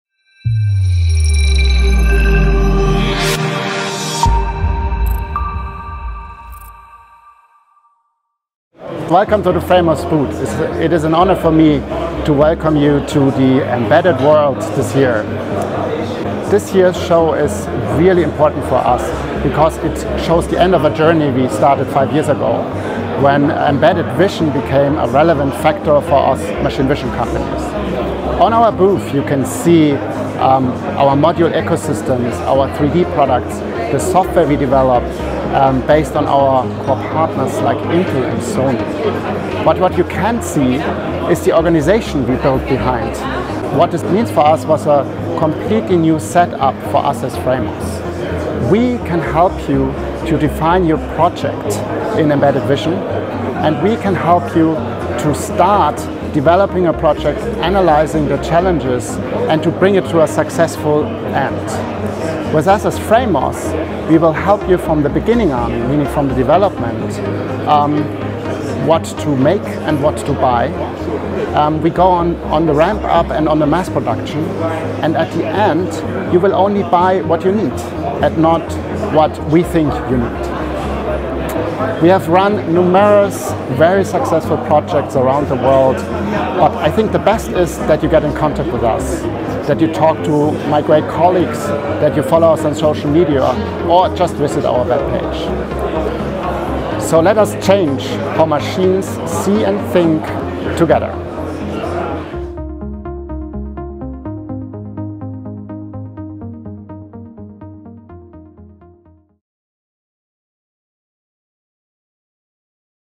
Welcome to our FRAMOS Virtual Exhibition | Dr. Andreas Franz
Embedded vision is an opportunity and paradigm shift for image processing companies like FRAMOS. This video explains how FRAMOS used this opportunity and could see this in action as you were guided through our booth at the 2020 Embedded World tradeshow.

Curious to learn more? We have prepared a virtual exhibition where we highlighted our demos from 2020 Embedded World tradeshow in Nuremberg.